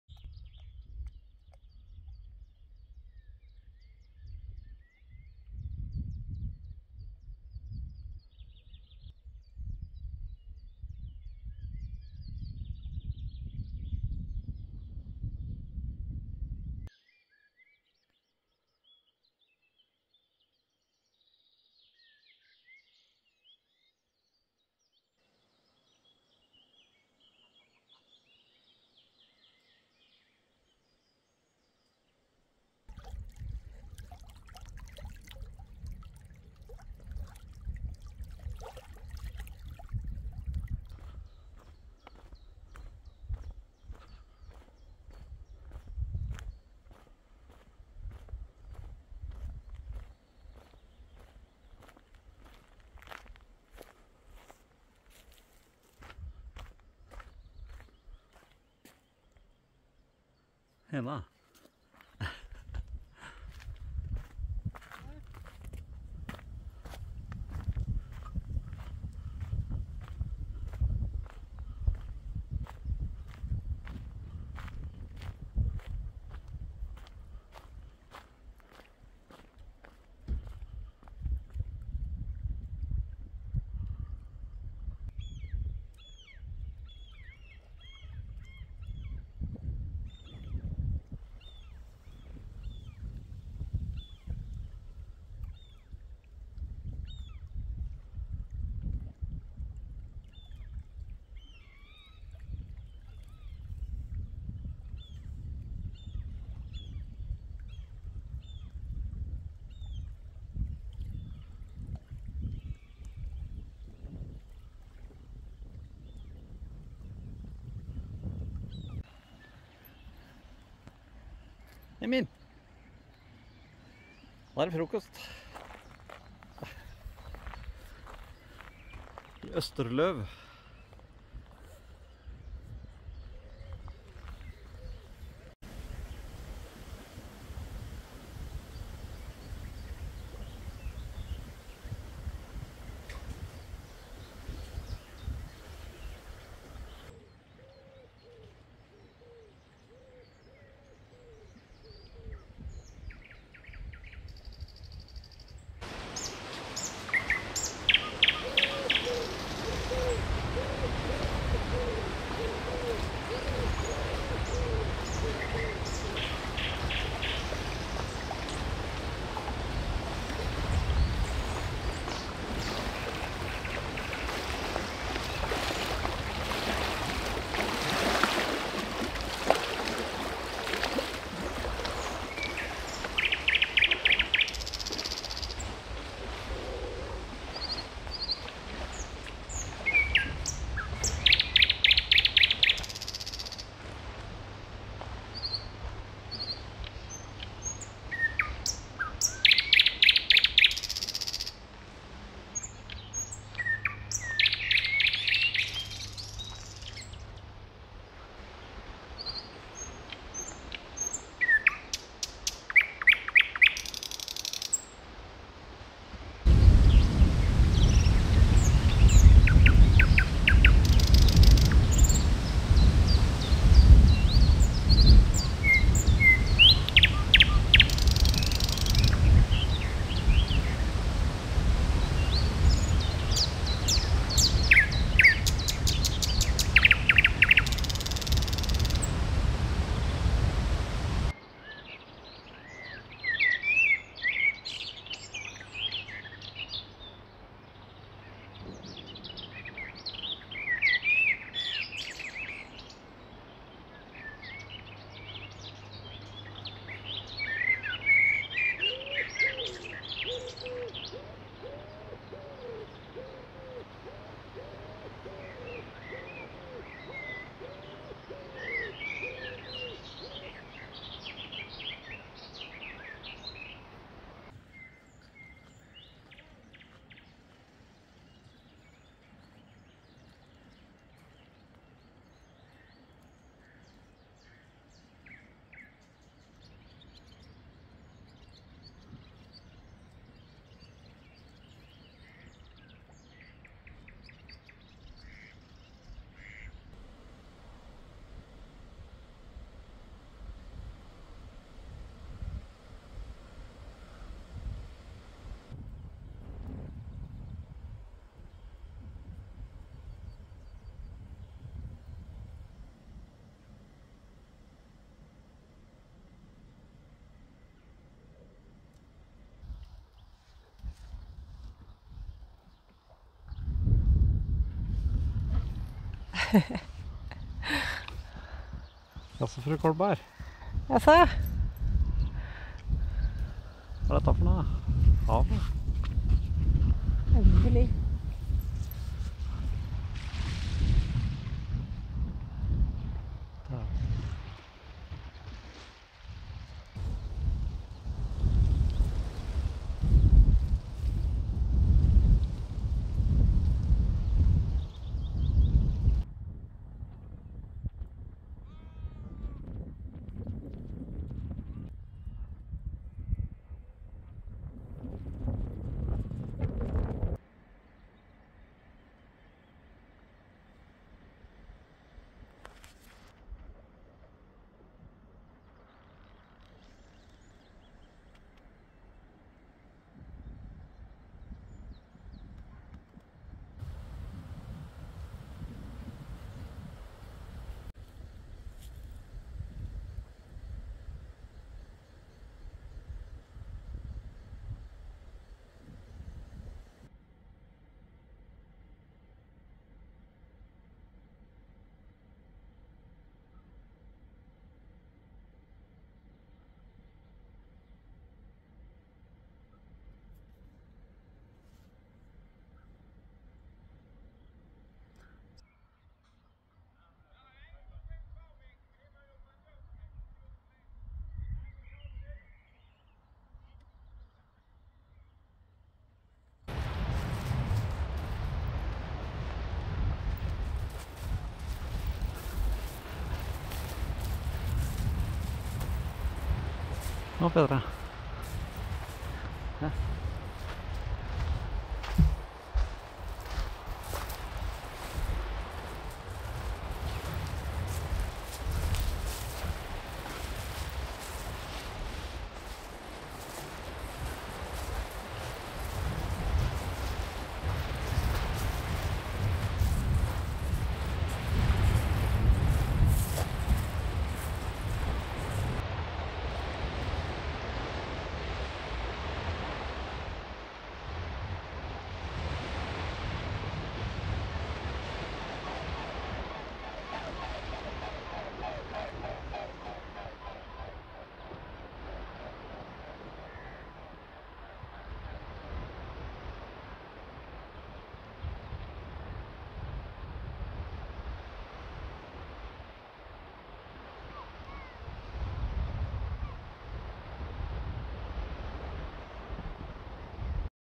From Lake Immeln to Österlen. Roadtrip Sweden. Day 5. May 26th 2023.
Nightingale should be: Thrush Nightingale  !